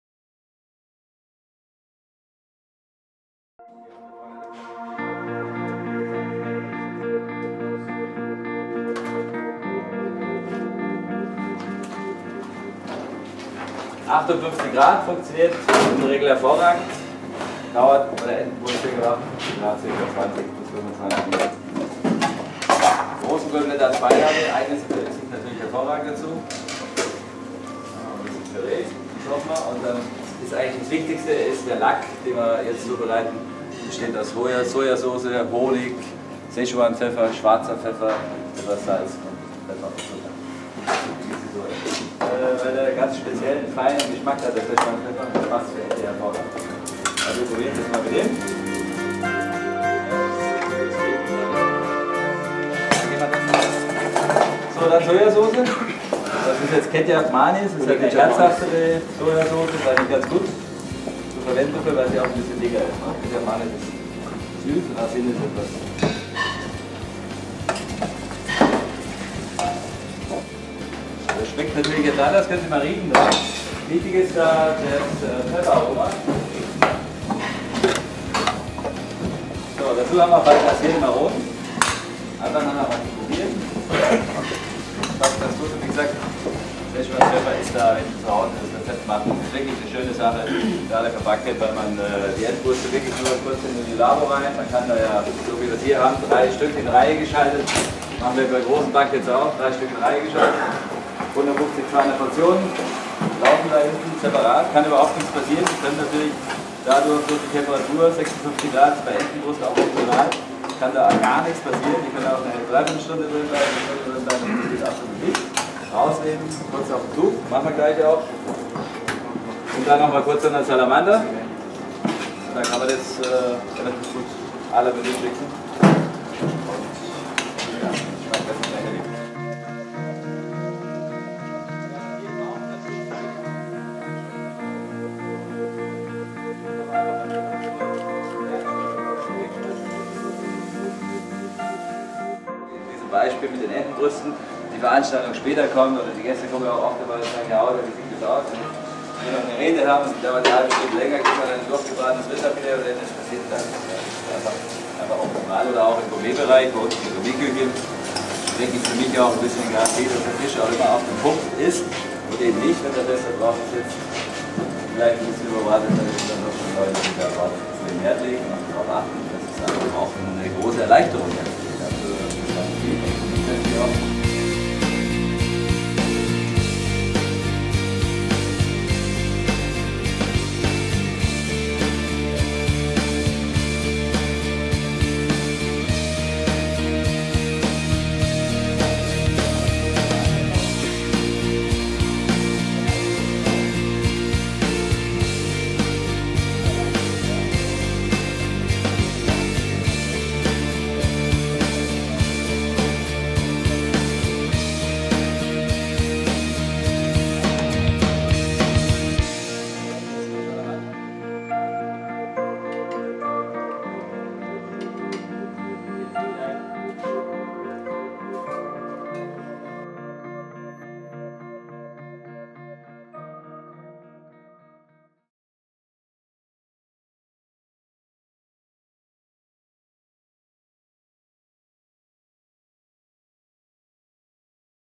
"Lackierte Entenbrust a`la Sous Vide" fusionchef Kochkurs mit Karlheinz Hauser
Sehen Sie hier den Auschnitt "Lackierte Entenbrust" aus dem Sous Vide Kochkurs mit dem Sternekoch Karlheinz Hauser (Süllberg Hamburg)